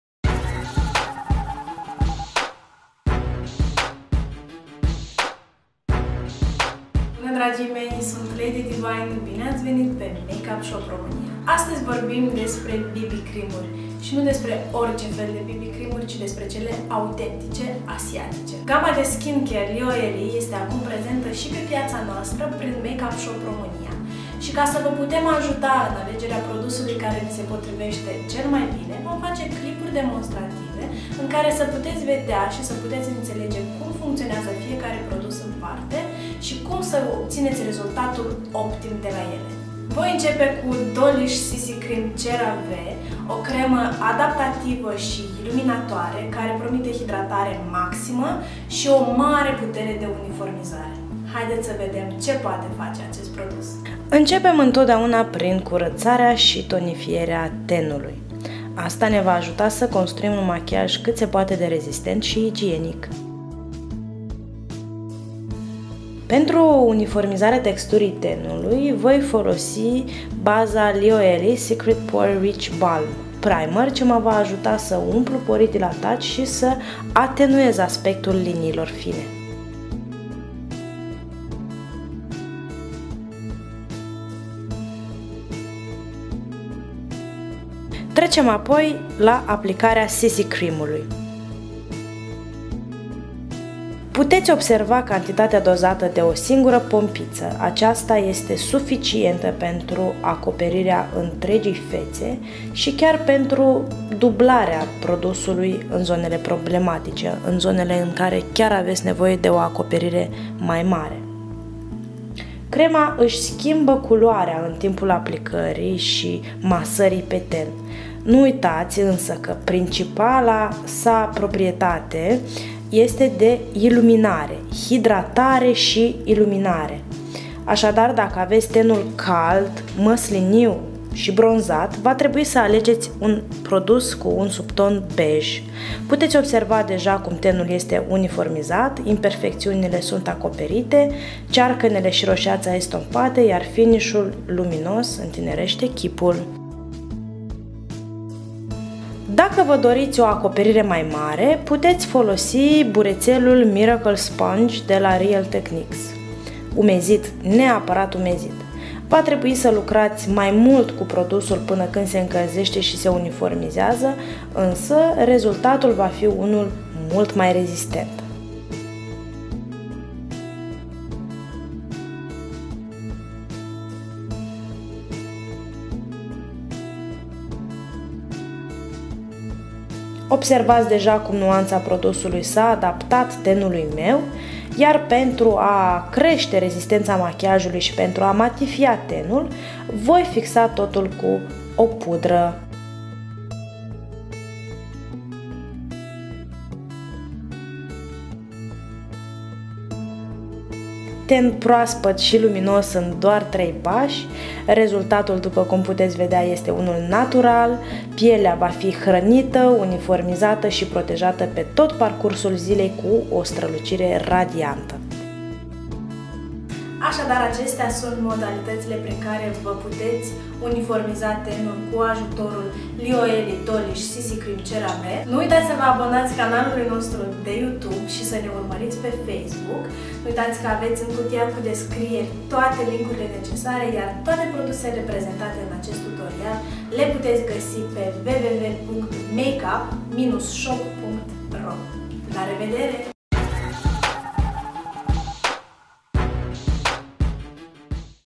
Lioele Dollish Cera-V CC
Produsele utilizate sunt:
Lioele Dollish Cera-V CC, SPF 34/PA++ este cel mai nou produs lansat in 2014 de liderul mondial in productie de BB Cream-uri si Skin care regenerativ - Lioele.
Acest CC Cream se adapteaza culorii tenului tau, il hidrateaza, corecteaza, il hraneste si il intinereste, il uniformizeaza si ii ofera protectie impotriva radiatiilor solare UV broad spectrum (UVA si UVB) avand un factor de protectie SPF 34.

Complexul de Ceramide, Vitamina E, extractele de Alge Marine, Baobab, Aloe Vera si uleiurile obtinute din seminte de jojoba si nuci de macadamia patrund adanc in piele, hranind si hidratand tenul pe parcursul intregii zile.

Produsul acopera petele, roseata, iritatiile, cearcanele si poate fi folosit ca baza / primer de fata pentru orice culoare de ten oferind acestuia o stralucire radianta, un aspect natural si rezistenta sporita machiajului.

Poate fi folosit pentru tipurile de ten cu subton rece.

Primer Lioele Secret Pore Rich Balm
Lioele Secret Pore Rich Balm este un primer premium ce reduce instant aspectul inestetic al porilor dilatati si al ridurilor.
Produsul controleaza timp indelungat secretia excesiva de sebum, matifiaza tenul si creste rezistenta in timp a machiajului. In plus, formula inovatoare HD contine elementele speciale ce difuzeaza lumina pentru rezultate excelente in conditii de filmare sau fotografie de inalta rezolutie.
Dupa utilizarea produsului, tenul devine mult mai elastic, mai neted si mai catifelat, iar aplicarea fondului de ten sau al BB Cream-ului este mult mai usoara.

Se recomanda aplicarea primerului, in special, in zonele cu ten gras.

Pentru testare produsul este disponibil si in varianta de pliculete mostra sau in varianta mini de 5g.

Extra Etichete: "